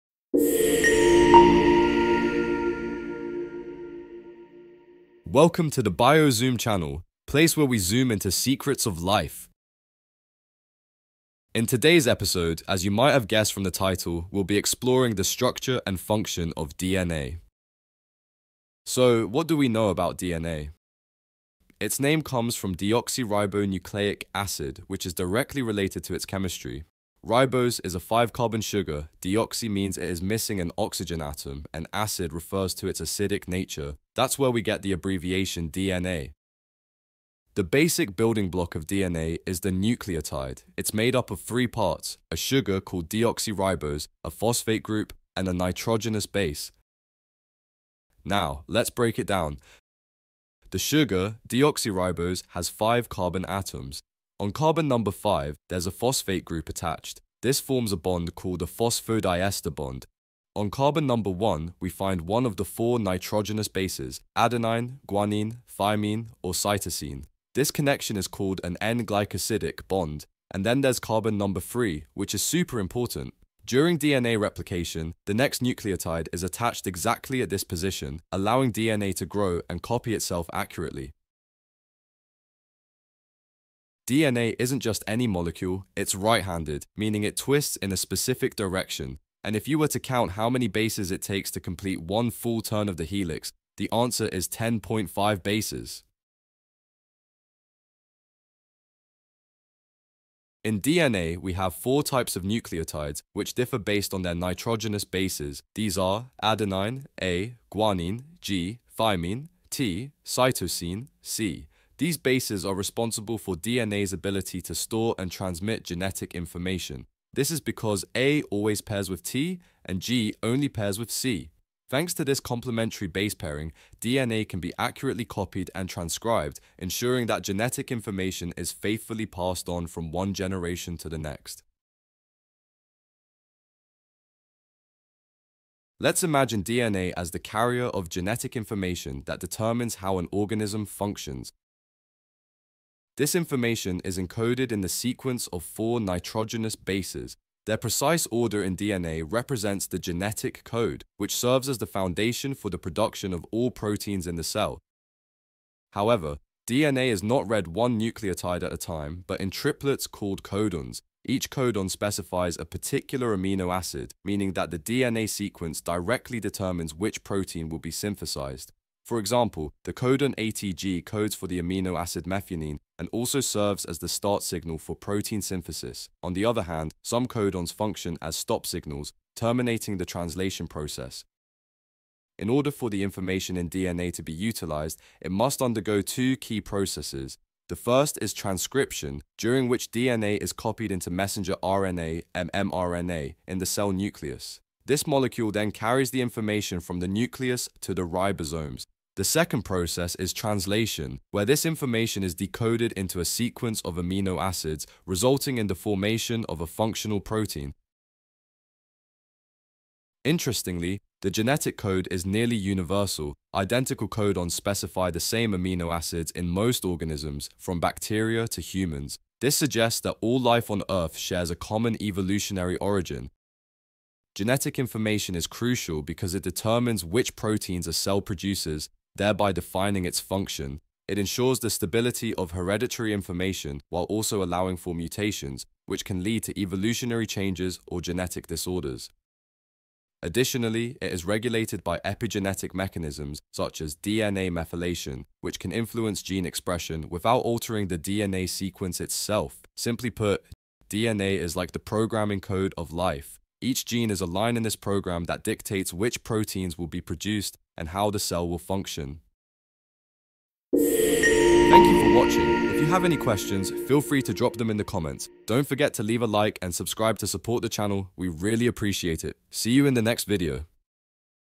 Basics of molecular biology E2 - DNA
In this video, we dive into the fascinating world of DNA! What does its structure look like? Why does it take the shape of a double helix? And how does it store genetic information? We’ll break down the fundamental unit of DNA—the nucleotide—its composition, and how DNA replicates and transmits the information essential for life.

You'll learn how the four nitrogenous bases (A, T, G, C) form the genetic code, the role of transcription and translation in protein synthesis, and why DNA is almost a universal code for life.